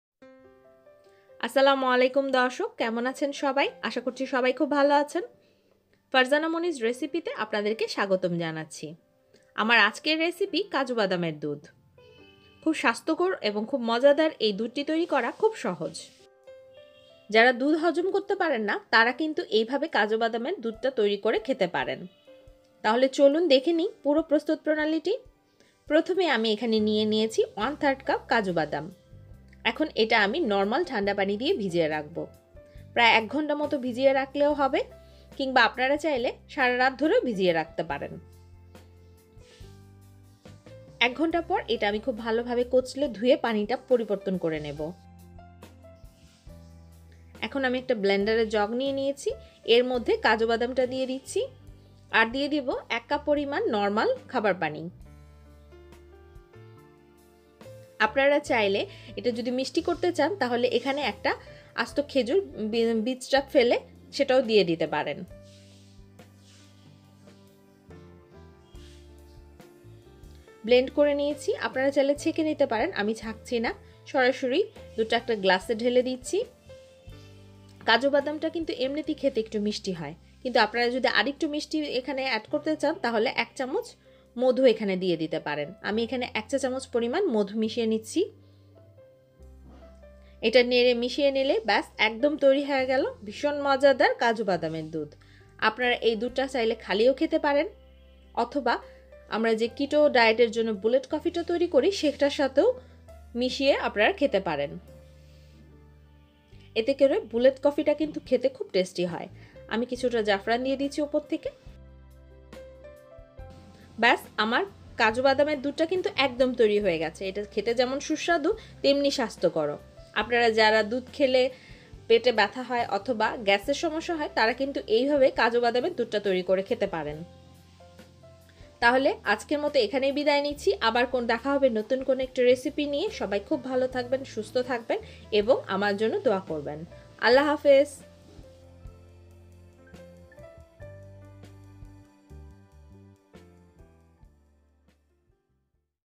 কাজুবাদামের দুধ || Cashew nut milk
আসসালামু আলাইকুম দর্শক।

আমার আজকের রেসিপি কাজুবাদামের দুধ।

ভীষণ সুস্বাদু এই রেসিপি টি আপনারা কিটো ডায়েট এ

ব্যবহার করতে পারবেন।